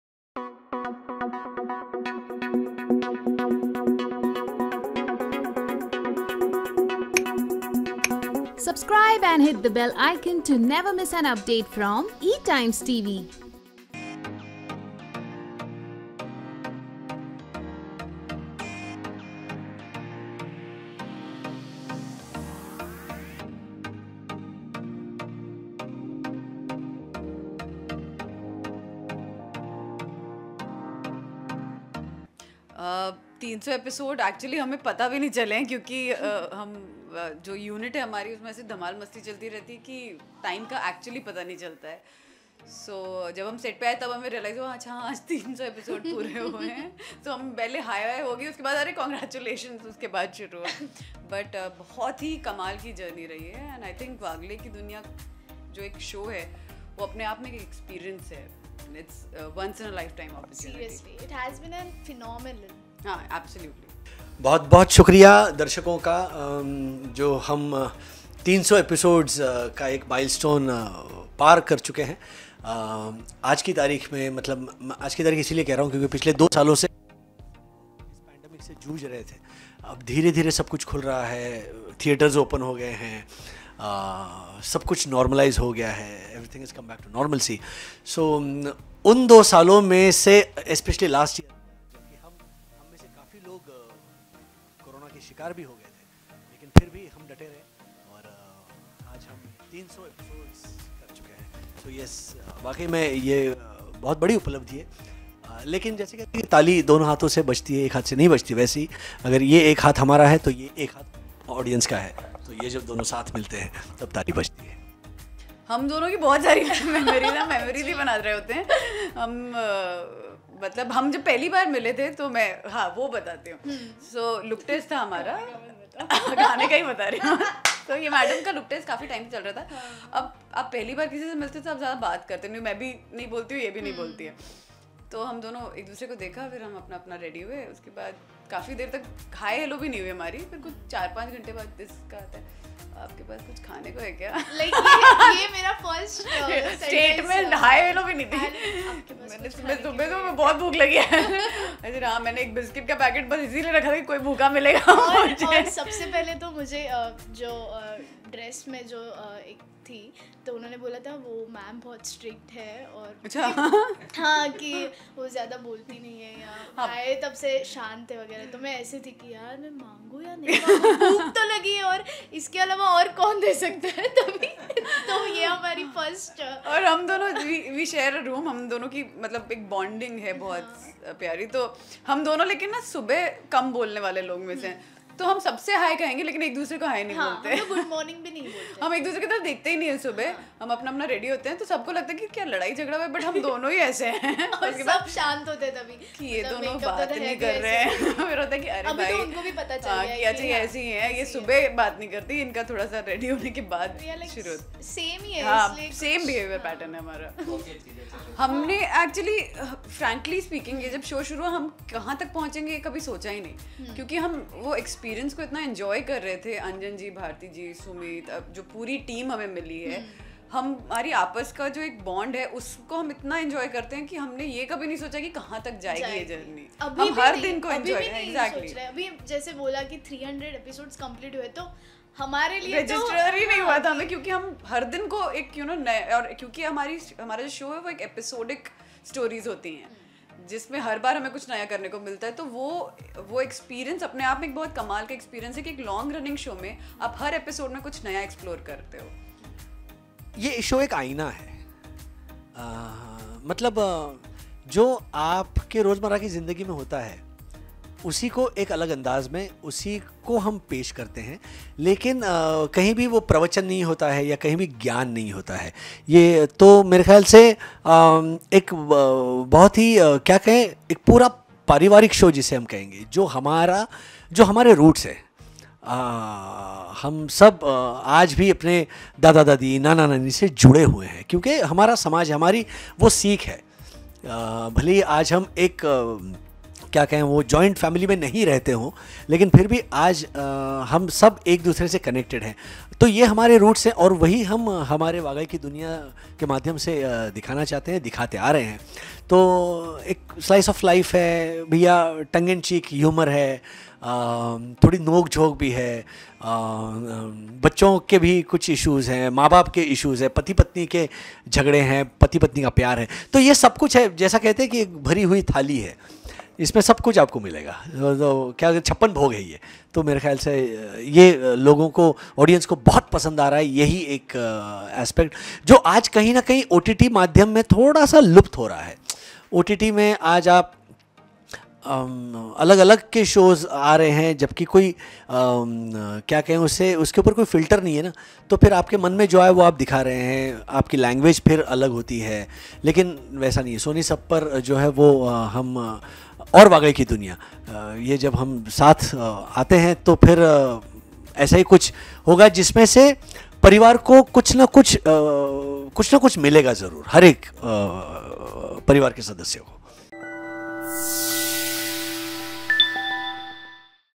Senior Wagles cut cake as Wagle Ki Duniya completes 300 episodes; team rejoices
Wagle Ki Duniya has crossed the milestone of 300 episodes. On the occasion, actors Sumeet Raghvan, Pariva Pranati and Chinmayi Salvi joined ETimes TV to share their feelings on the same. Exhilarated about this achievement, the cast shared how surprising it was for them to learn that they’ve had a journey this long. As they recalled their time on the sets, they also shared a few most memorable moments they’ve lived together. Sumeet also spoke about the special aspects of the show and said, “Ye show chappan bhog hai, isme aapko sab kuch milega. This is missing from the OTT shows.”